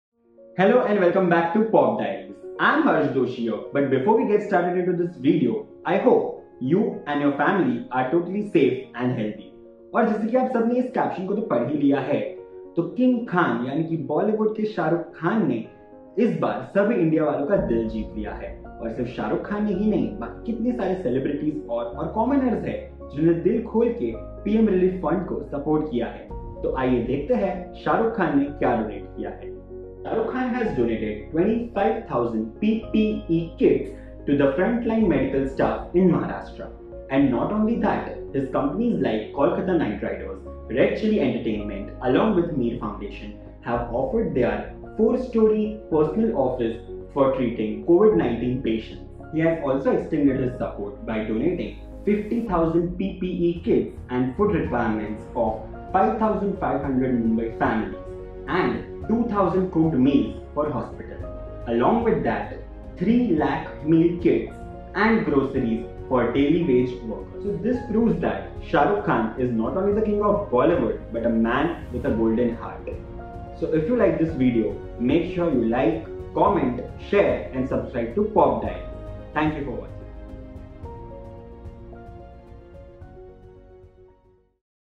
Shah Rukh Khan's GENEROUS Donation For Fight Against Covid19
Shah Rukh Khan is not just king of romance but he is also large hearted king who has a generous heart when it comes to charity. SRK has done multiple contribution to help the government fight against covid19, find it out here.